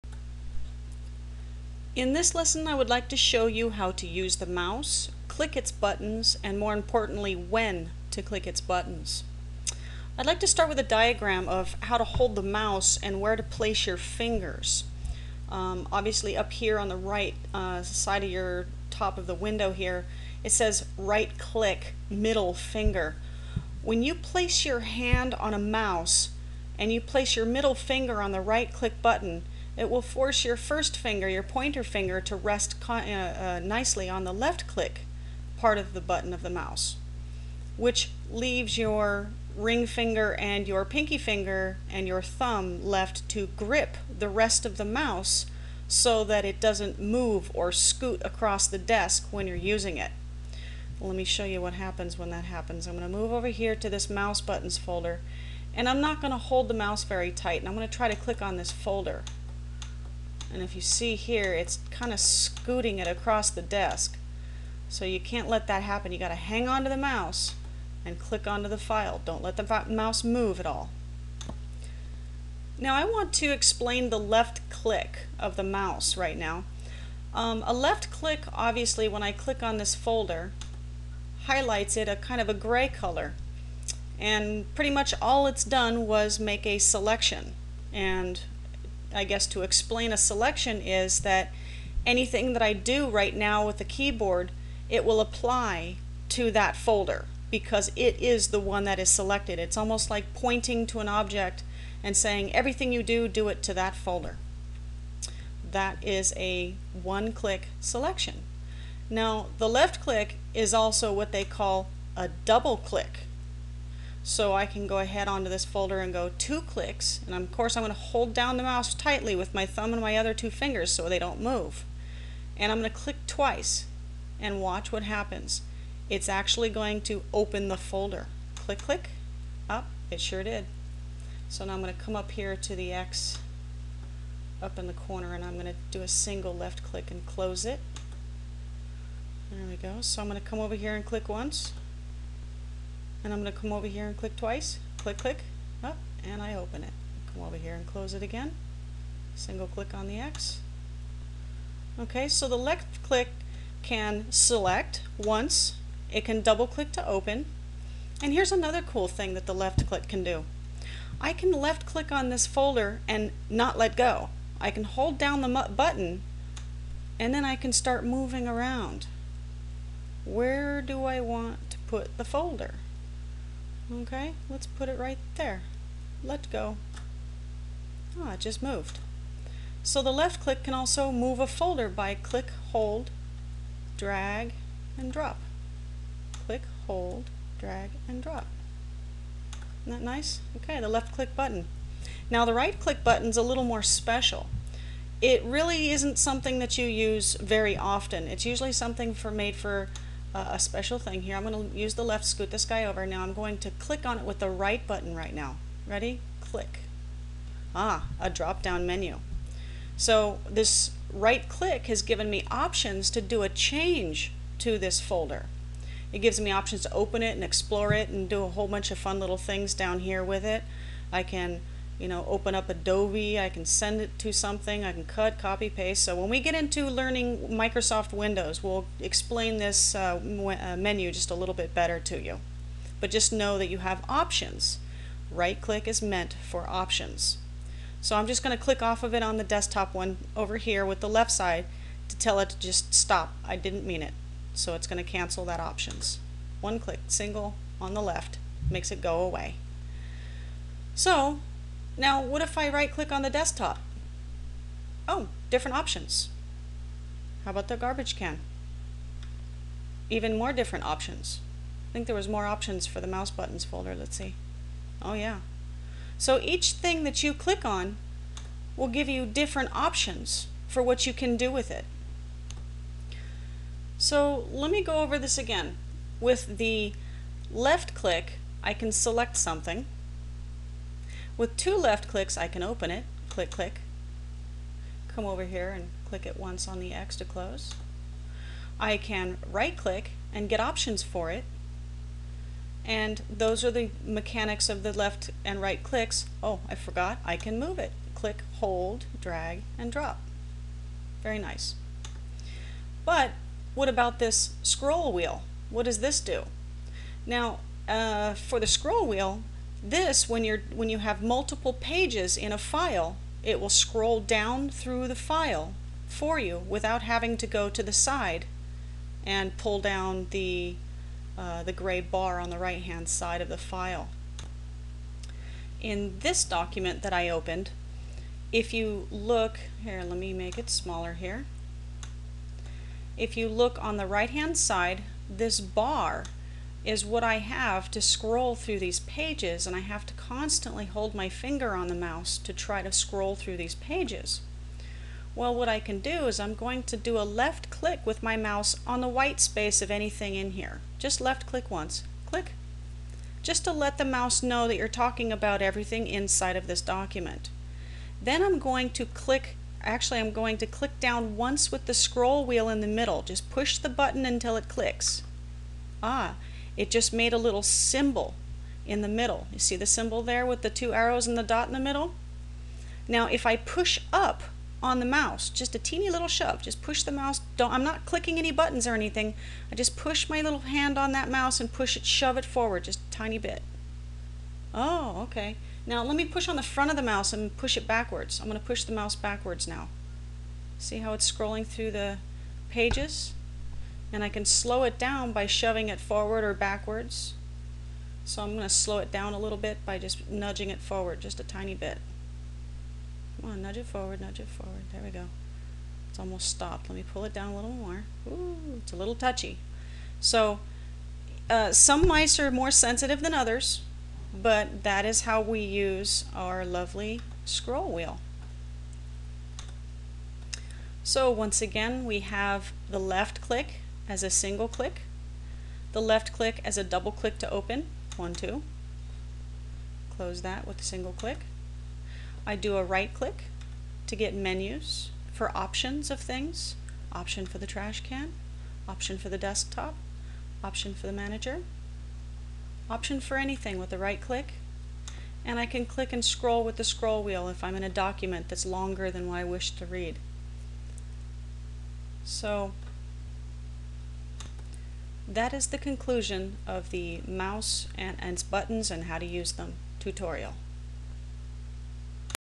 Using the mouse: Tutorial for seniors
A Slow and concise tutorial on how to use the mouse buttons, what they do, and when to click them.